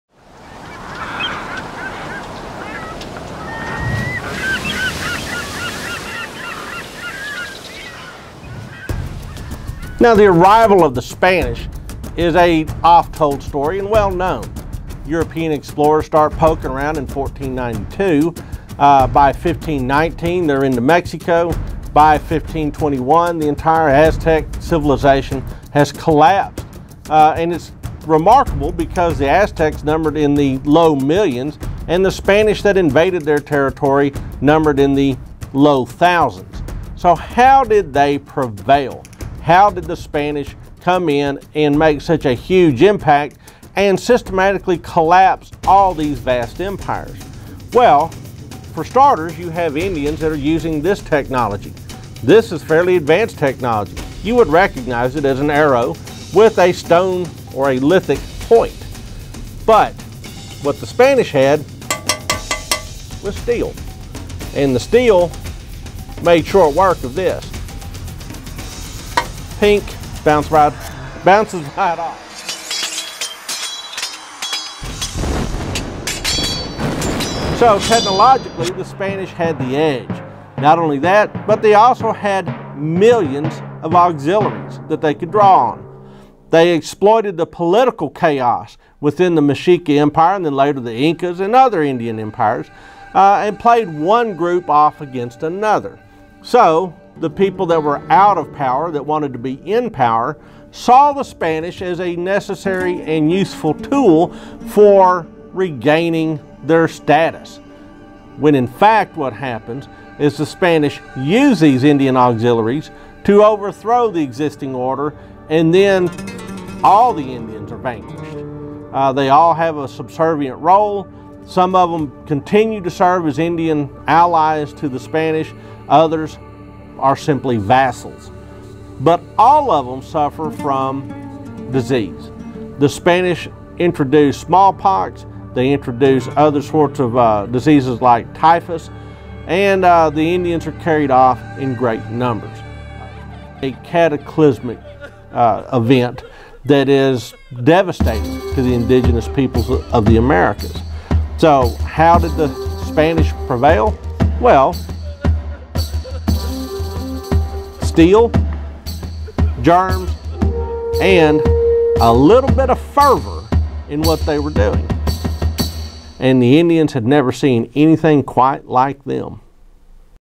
L6V3 Fall of the Meso American Empires
When worlds collide the history of humankind will change forever.  The indigenous people of North America would never recover.